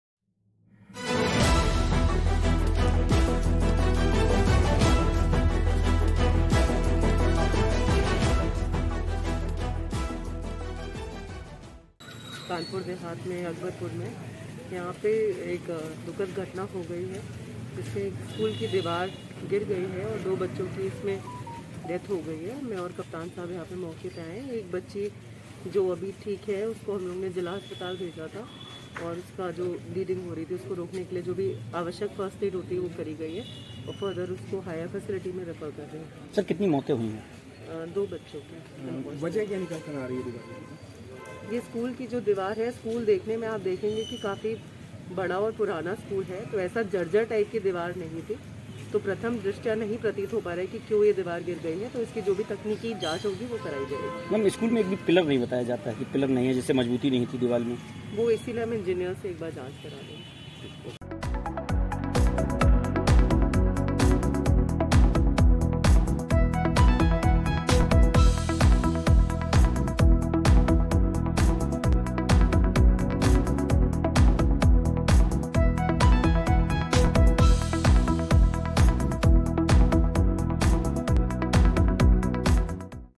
Kanpur News: कानपुर देहात में इस स्कूल की दीवार गिरने से हुई दो बच्चों की मौत..
Kanpur News: कानपुर देहात में स्कूल की जर्जर दीवार गिरने से दो बच्चों की मौत हो गई. वहीं एक बच्ची बुरी तरह घायल हो गई.